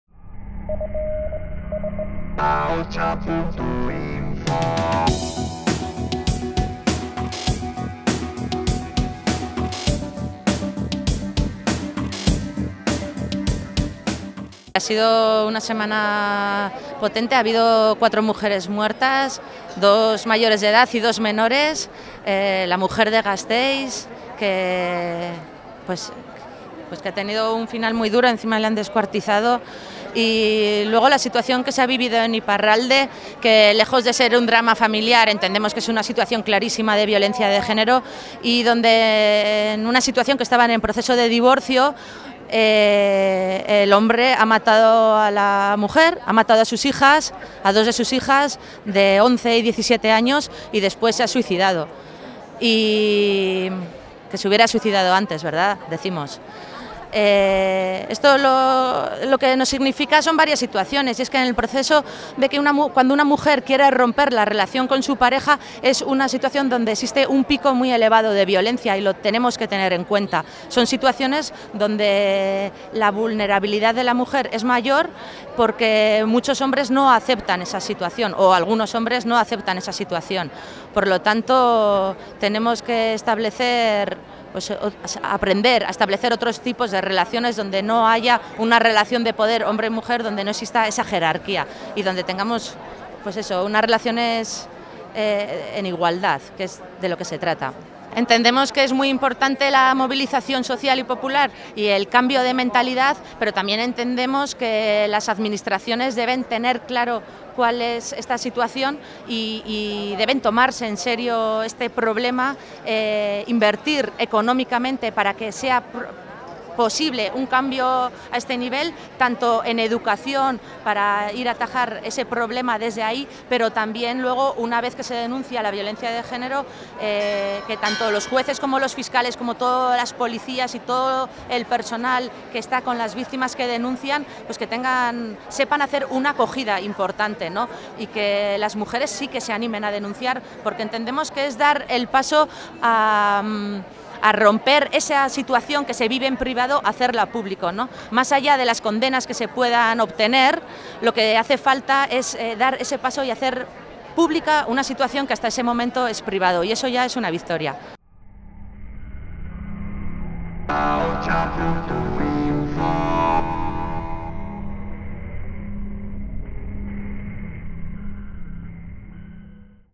Concentración contra los últimos asesinatos machistas en EH
Concentración en Iruñea para denunciar los últimos asesinatos machistas en Gasteiz y Angelu.